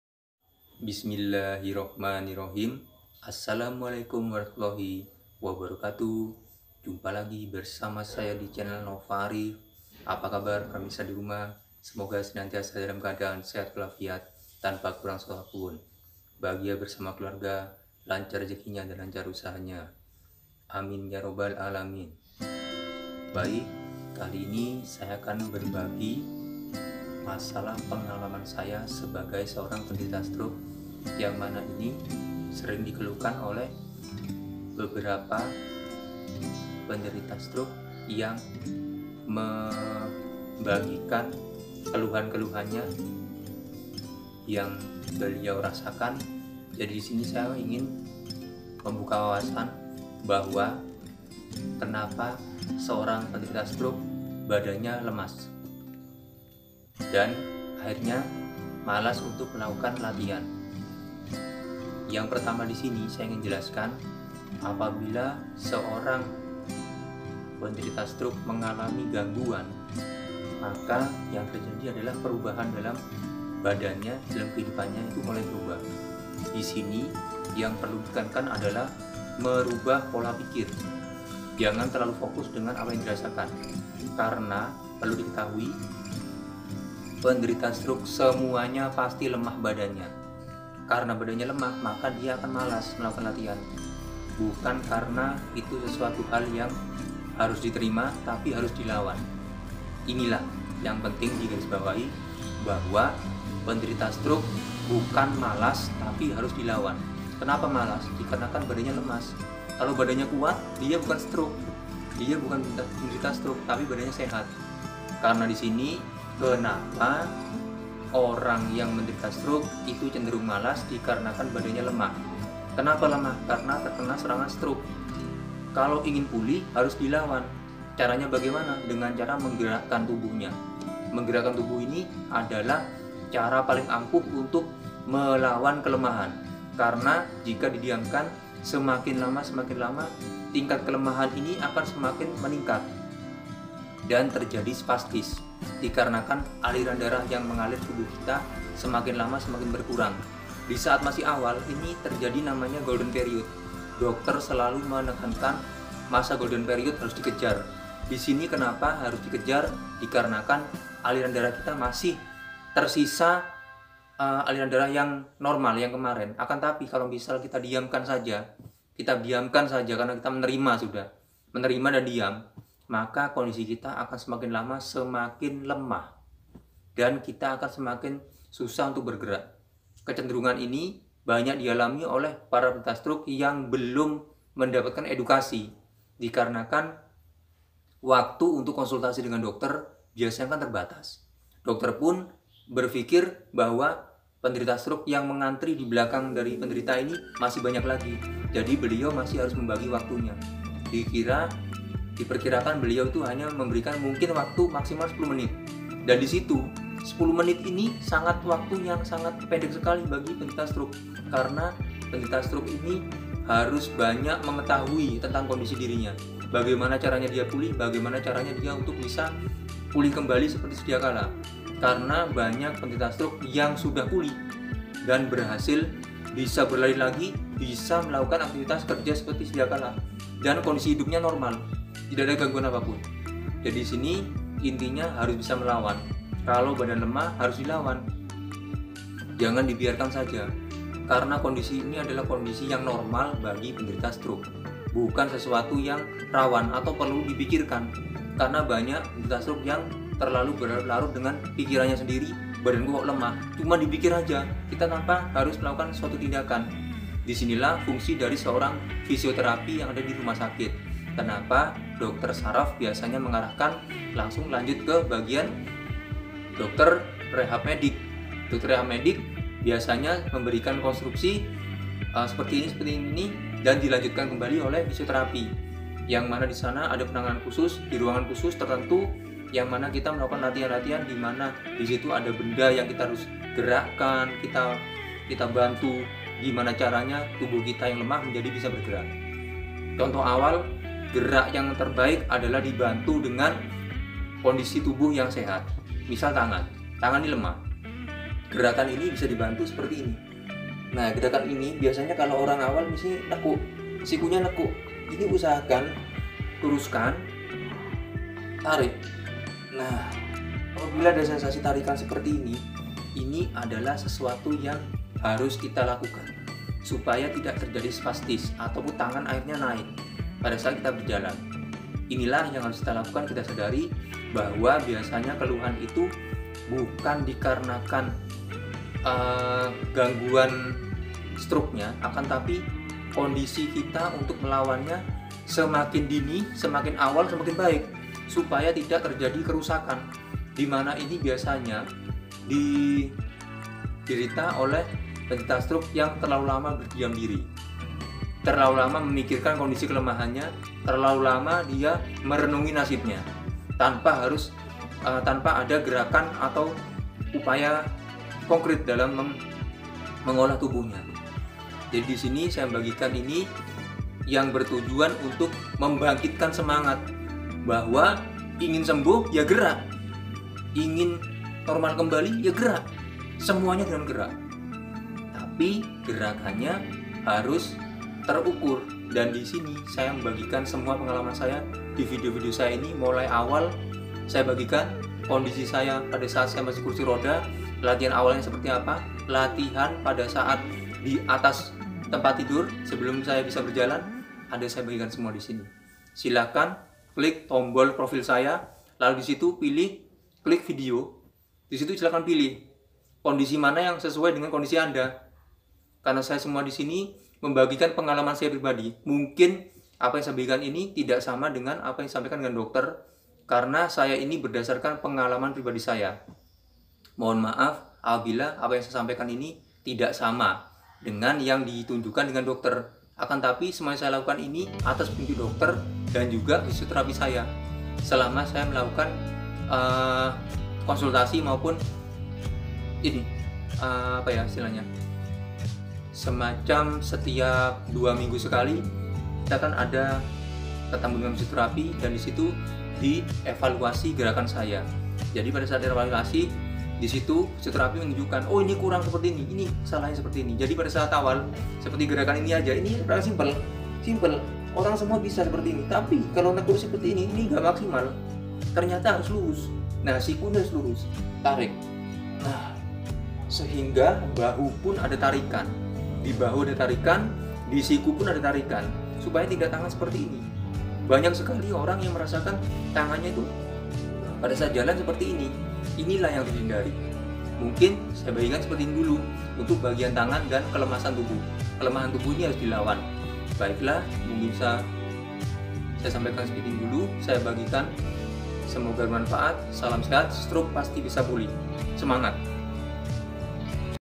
MENGAPA BADANKU LEMAH DAN MALAS UNTUK BERGERAK???
Halo saya Nova Arif video ini saya bagikan berdasarkan pengalaman pribadi saya sebagai seorang pendeta stroke yang mengalami kelemahan tubuh akibat serangan stroke semoga apa yang sambungkan ini bisa berikan motivasi dan semangat tambahan bagi para penderita stroke maupun keluarga yang berikan dukungan pada anggota keluarganya yang terkena serangan stroke semoga bermanfaat salam sehat assalamualaikum morfologi wabarakatuh stroke pasti bisa pulih amin ya robbal alamin